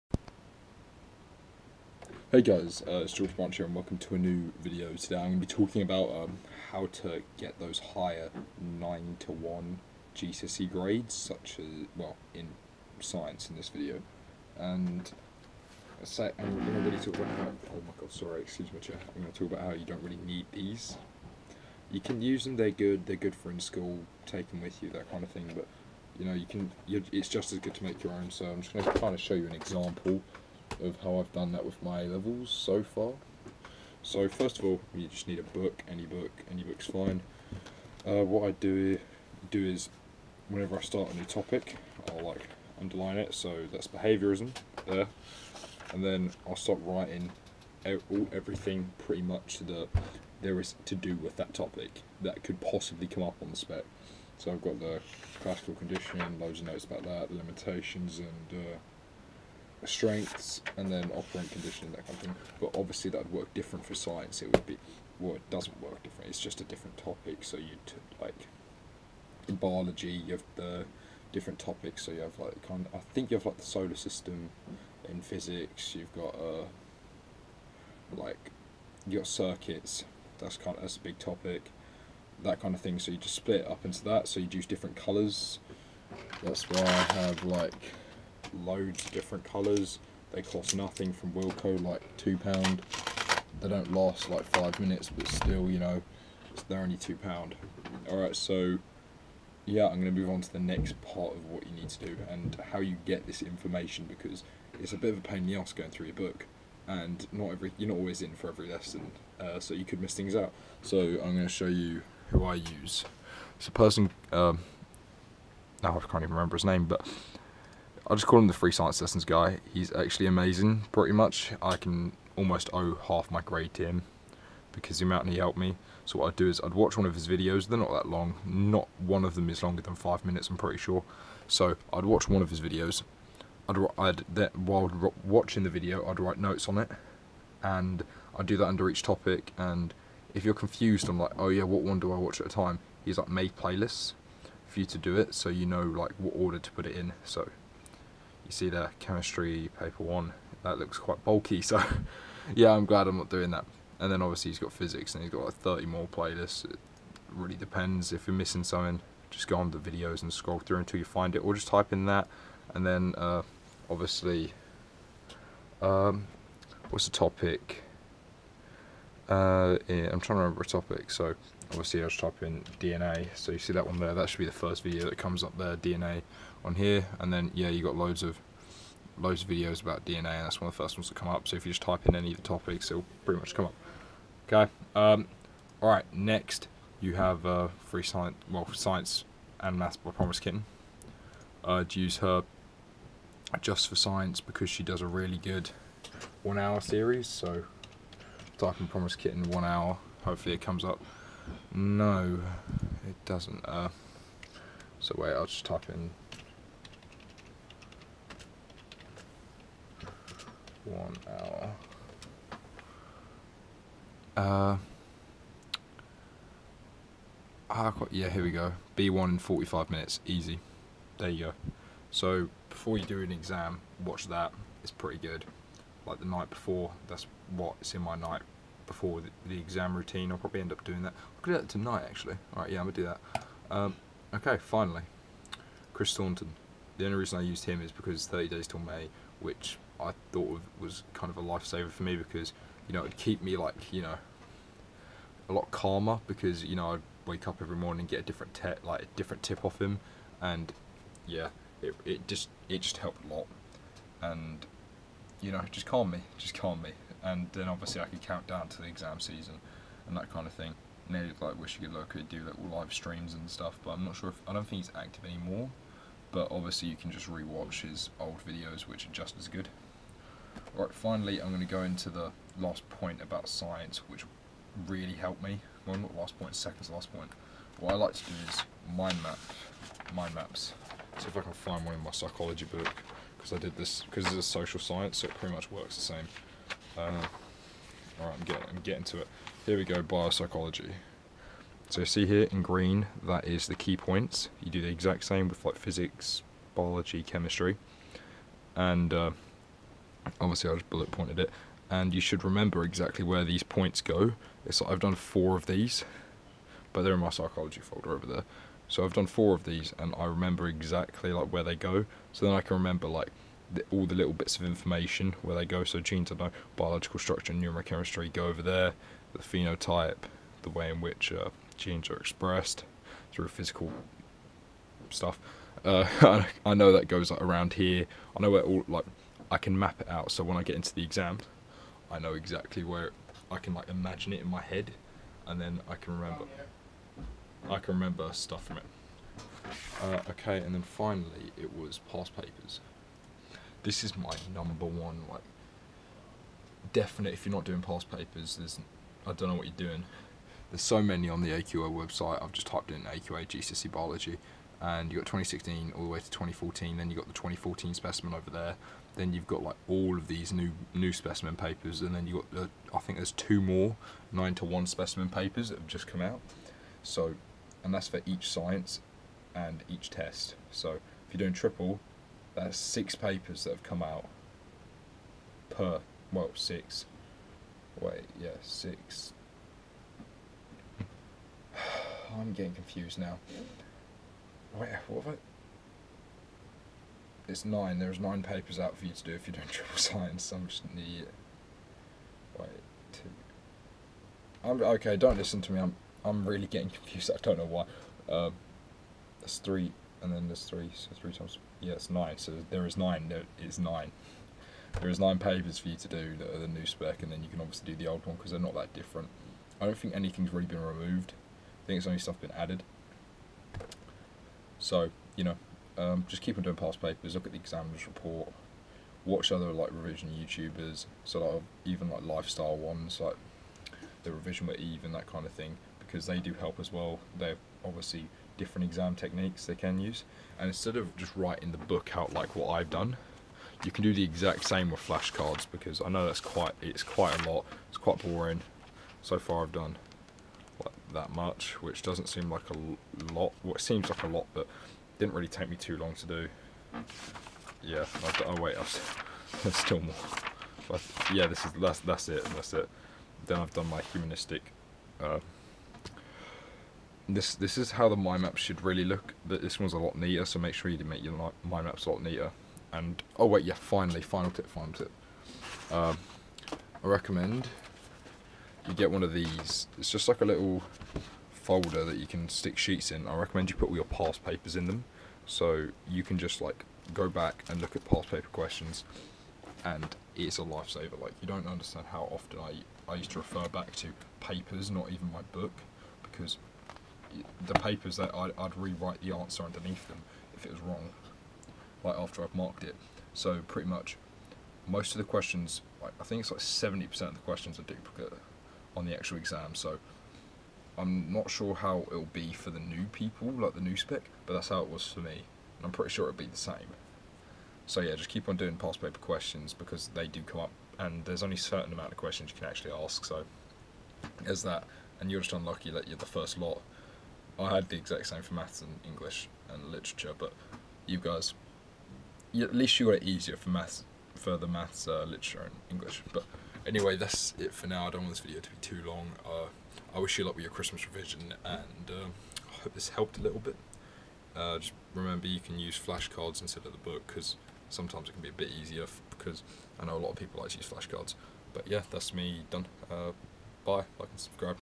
How To Revise GCSE Science For Grades 7-9 (A-A*)
Hey my names George im 17 and i just started college and i love playing rugby, going out, going to the gym and socialising.
Social Medias
General Comments
I know i stuttered quite a lot in this video but hopefully i got my general tips out across.
Also i did forget about flash cards i did use them occasionally and they can replace using the book and have the exact same effect but i just kept losing them so stuck to a book.